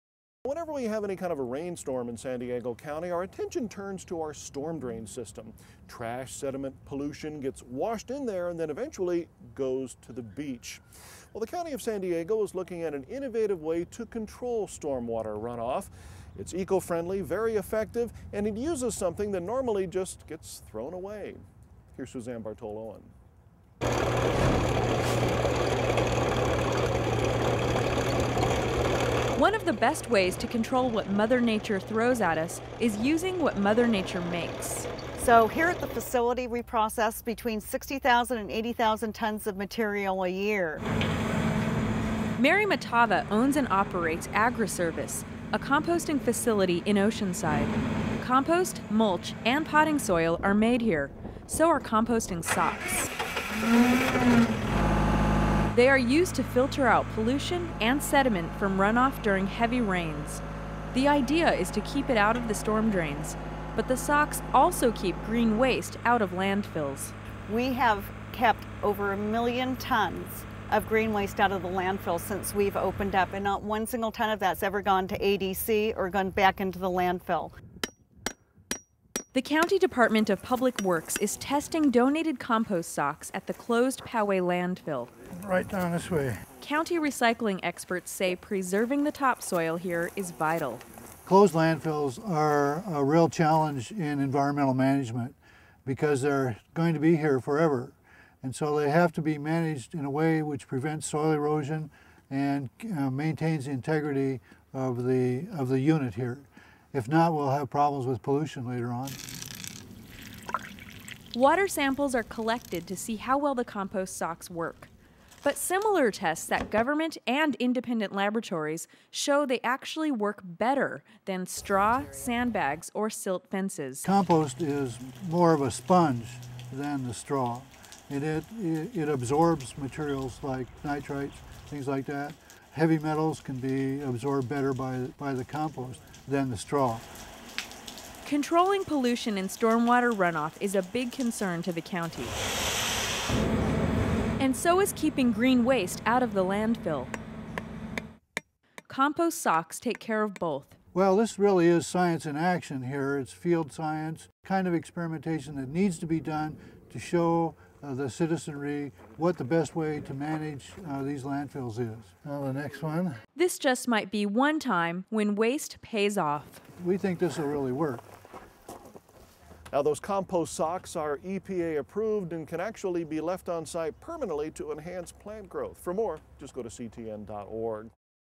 Science in Action-Compost Wattles
The County of San Diego is looking at an innovative way to control storm water runoff.  It's eco-friendly, it's effective and uses something that is usually thrown away.